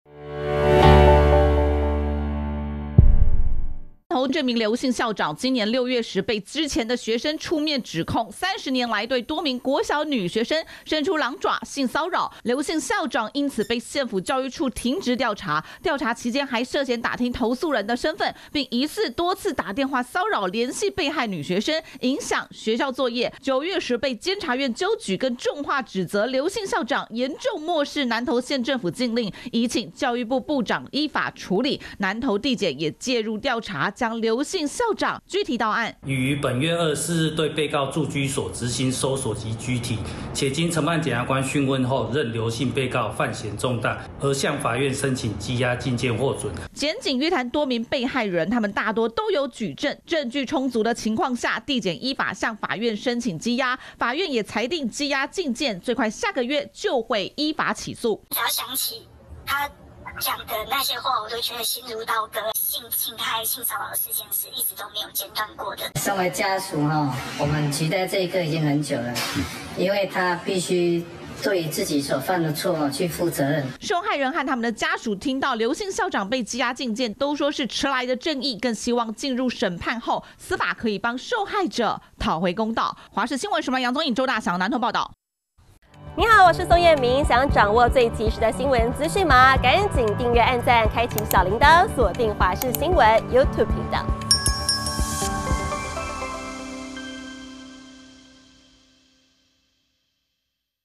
台版熔爐案！涉性騷女童犯行30年 惡狼校長收押禁見｜華視新聞 20231027
南投一名劉姓國小校長被指控30年來對多名國小女學生性騷擾、妨害性自主，調查期間還涉嫌打聽投訴人身分，干擾調查，9月時被監察院糾舉，南投地檢署也因此將劉姓校長拘提到案，訊問後認為他涉嫌重大，並且有湮滅、變造證據或勾串證人之虞，向法院聲請羈押獲准。